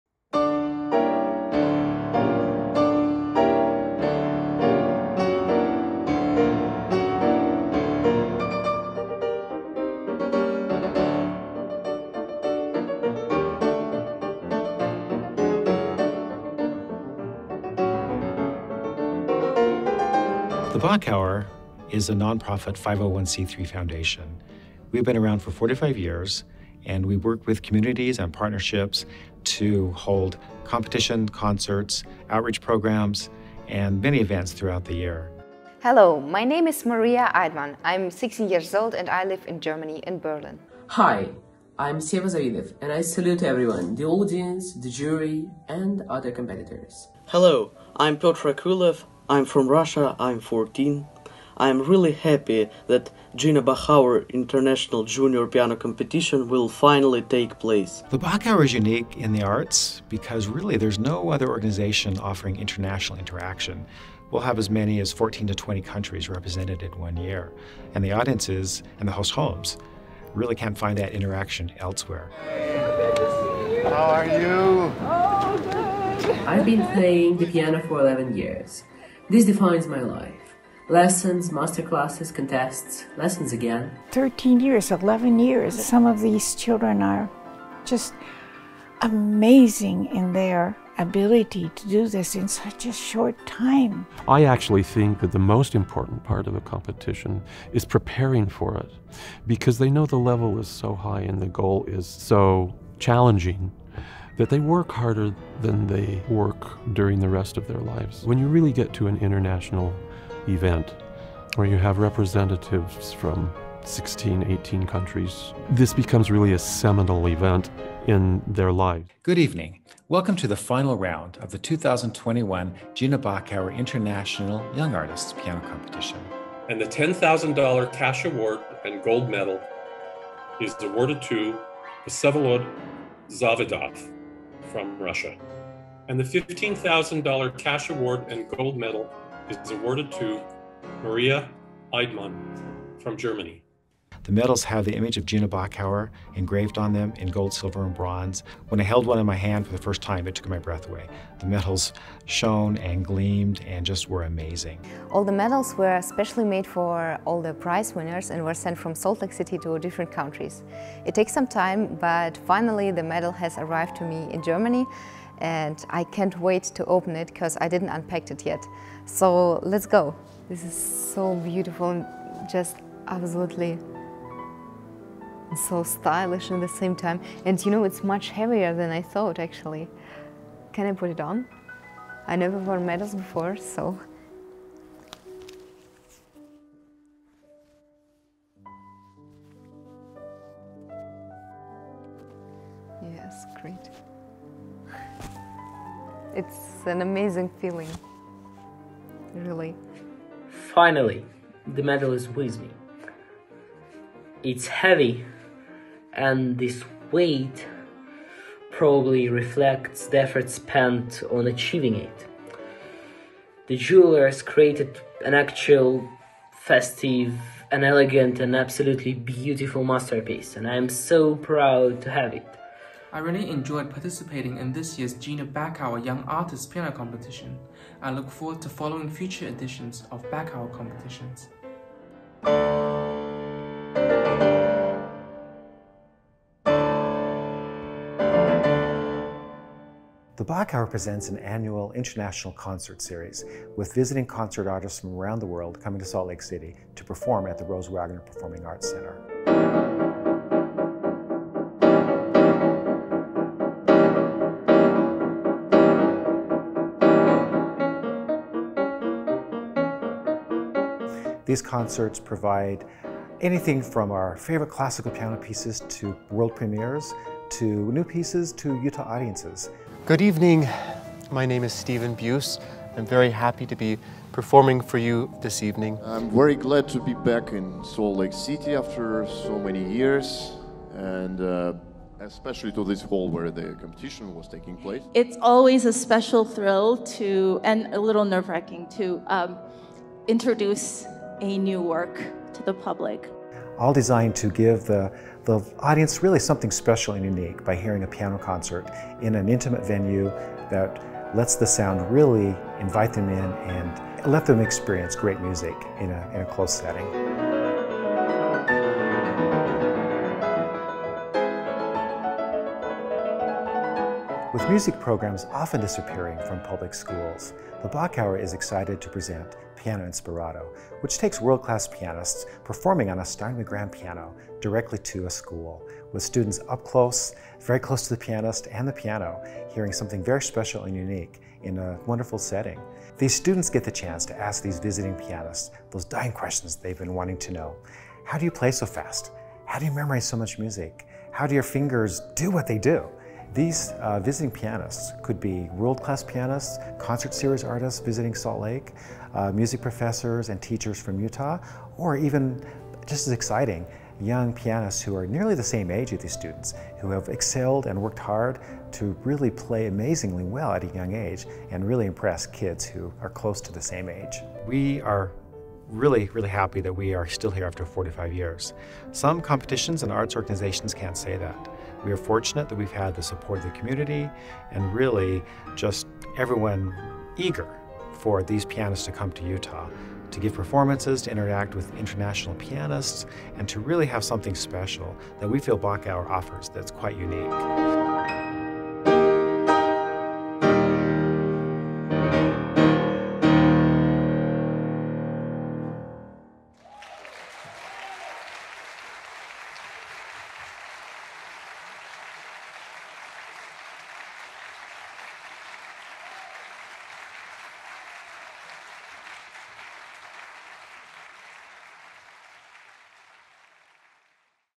2021 22 Bachauer Foundation Highlights
Highlights from the Gina Bachauer International Piano Foundation's 2021-22 Artistic Programs, including:

2021 Gina Bachauer International Junior Piano Competition
2021 Gina Bachauer International Young Artists Piano Competiiton
2021-22 Gina Bahcauer Internaitonal Piano Concert Series
2022 Gina Bachauer International Piano Festival
2021-22 Piano Inspirato Outreach Programs